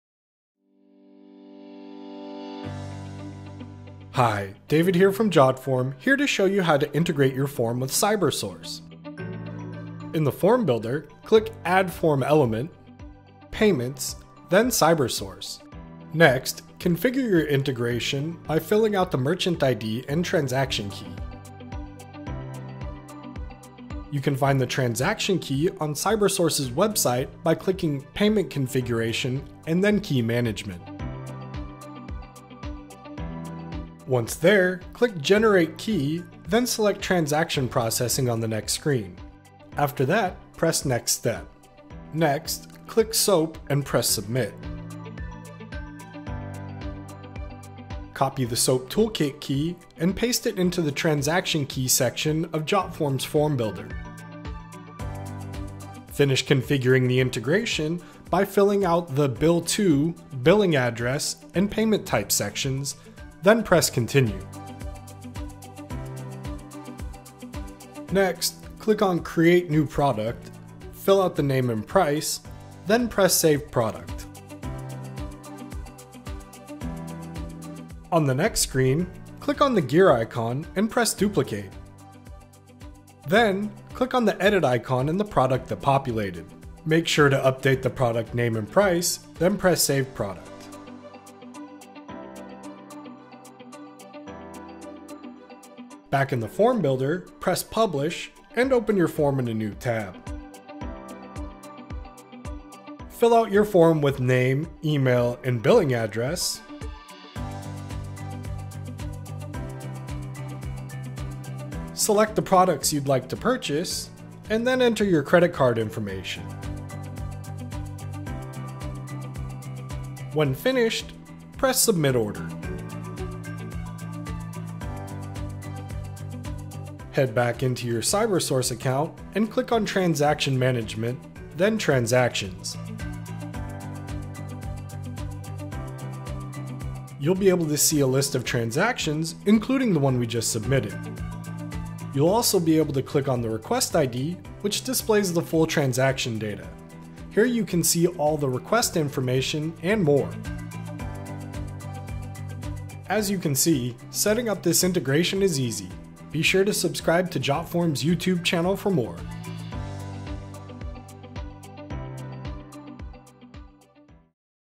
How to integrate Jotform with CyberSource
Visa subsidiary CyberSource simplifies online and in-person payments for small businesses in more than 190 countries. Learn how to set up Jotform's integration with the CyberSource payment gateway in just a few clicks!
👋 ABOUT JOTFORM
Hi, we're Jotform, a full-featured online forms platform that makes it easy to create robust forms and collect important data. Check us out
TRANSCRIPTION

In the Form Builder, click “Add Form Element,” “Payments,” then “CyberSource.”

Next, configure your integration by filling out the Merchant ID and Transaction Key.

You can find the Transaction Key on CyberSource's website by clicking “Payment Configuration,” and then “Key Management.”

Once there, click “Generate Key,” then select “Transaction Processing” on the next screen. After that, press “Next Step.”

Next, click “SOAP” and press submit.

Copy the “Soap toolkit key” and paste it into the “Transaction Key” of Jotform’s Form Builder.

Finish configuring the integration by filling out the “Bill to,” “Billing Address,” and “Payment Type” sections. Then press “Continue.”

Next, click “Create New Product.” Fill out the name and price. Then press “Save Product.”

On the next screen, click on the “gear” icon and press, “duplicate.”

Then, click on the “edit” icon in the product that populated.

Be sure to update the product name and price. The press “Save Product.”

Back in the Form Builder, press “Publish” and open your form in a new tab.

Fill out your form with name, email, billing address, product you’d like to purchase, and credit card information. Then press “Submit order.”

Head back into your CyberSource account and click on “Transaction Management,” then “Transactions.”

You’ll be able to see a list of transactions including the one we just submitted.

You’ll also be able to click on the “Request ID,” which displays the full transaction data. Here, you can see all of the “Request Information” and more.

As you can see, setting up this integration is easy!